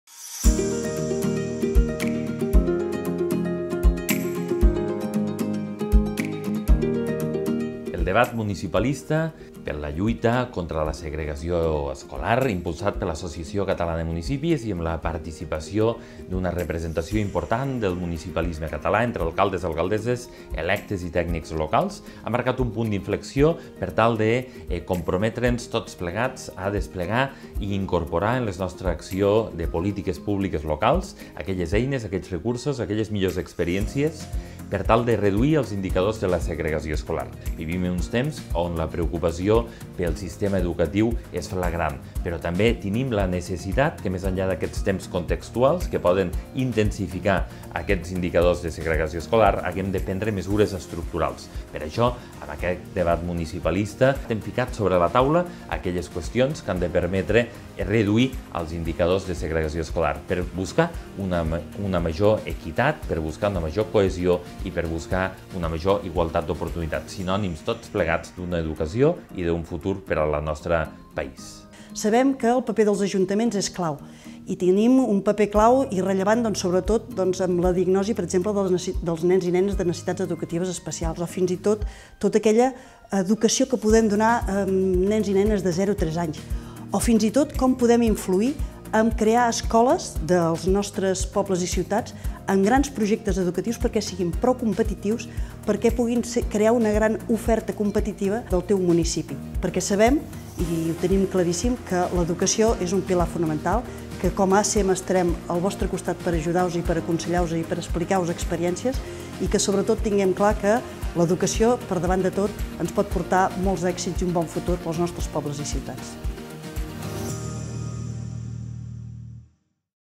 DebatMunicipalista​: 'El paper dels municipis en la lluita contra la segregació escolar'
El municipalisme català es fixa com a prioritat combatre la segregació escolar als pobles i ciutats de Catalunya

Lluís Soler: “Invertir en educació és invertir en futur, i el país que volem, cohesionat, equilibrat i ple d’oportunitats només serà possible si enfortim l’educació per fer un millor país per a tothom”.

Més de 100 persones entre alcaldes, regidors, tècnics locals i persones vinculades amb la comunitat educativa participen en un nou